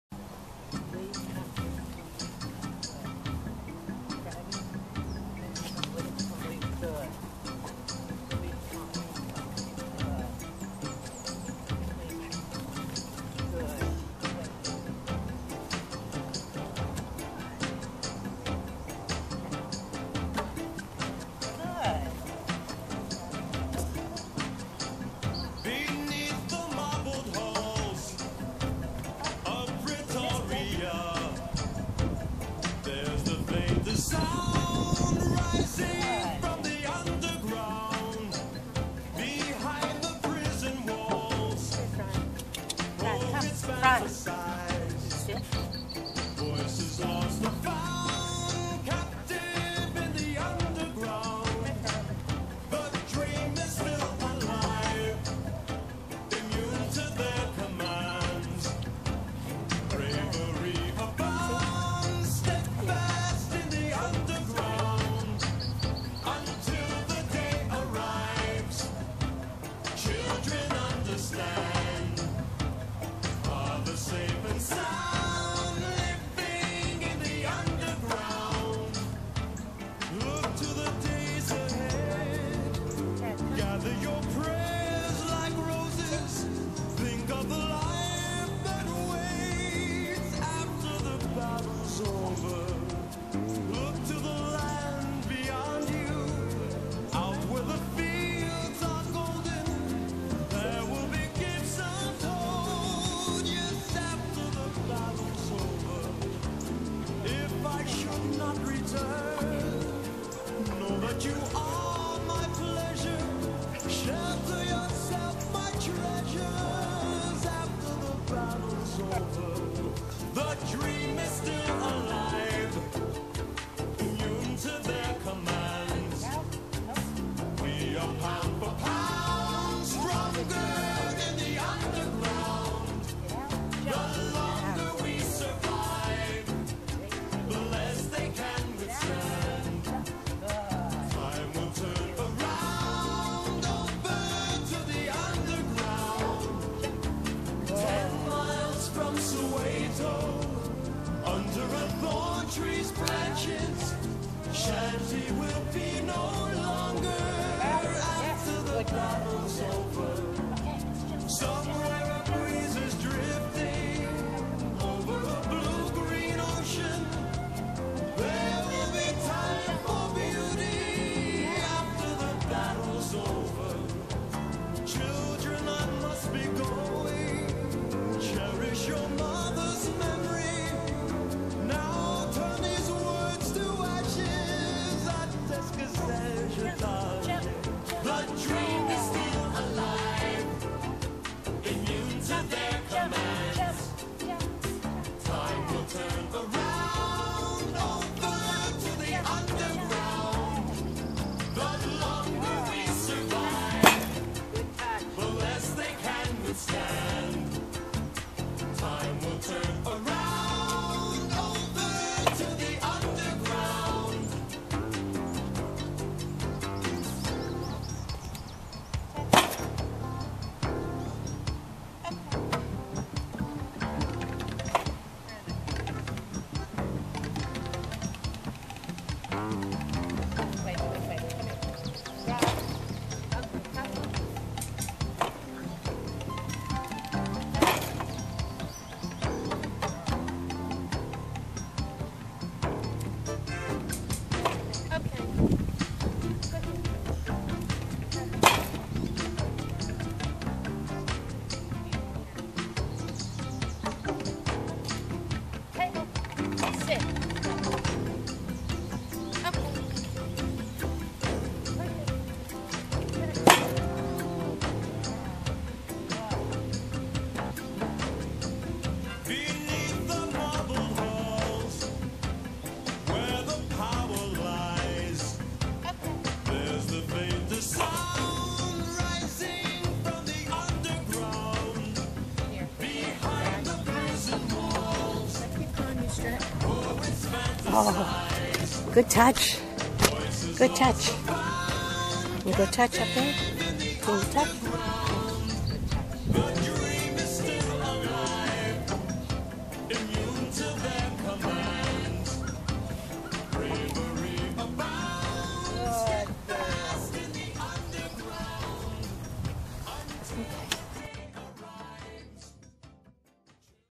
THRIVE training May 12, Mother's Day
A beautiful day. weather is great. temps are great and the lawn has dried out.
That makes for a great day for training all the dogs.  Here is Thrive.